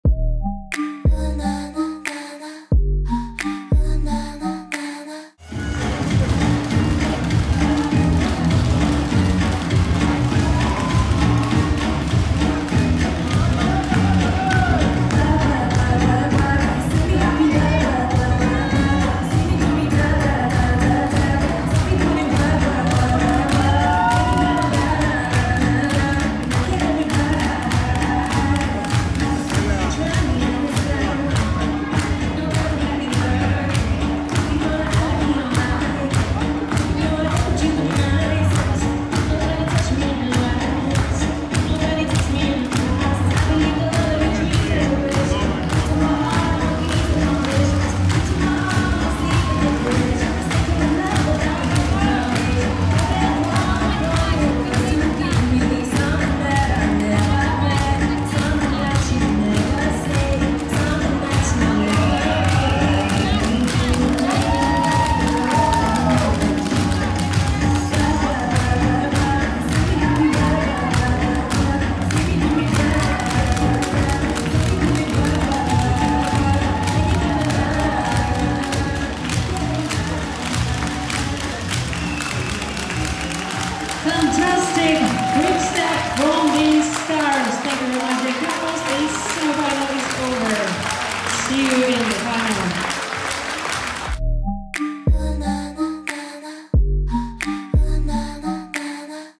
Marek Klepadlo & Zhiting Yang | Quickstep | Professional Standard, Superstar Dance Festival
Professional Open Standard Cup

Final:
1. Domen Krapez & Natasha Karabey - Germany
2. Andrii Mykhailov & Ekaterina Popova - USA
3. Marek Klepadlo & Zhiting Yang - USA
4. Igor Kobiuk & Valeryya Kalyschuk - Ukraine
5. Roman Sukhomlyn & Olga Gandembul - Ukraine
6. Maciej Nagorski & Magdalena Fron - Poland

Semi-final:
7. Igor Colac & Roxane Milotti - USA
8. Nazar Goncharenko & Marina Tuzova - Ukraine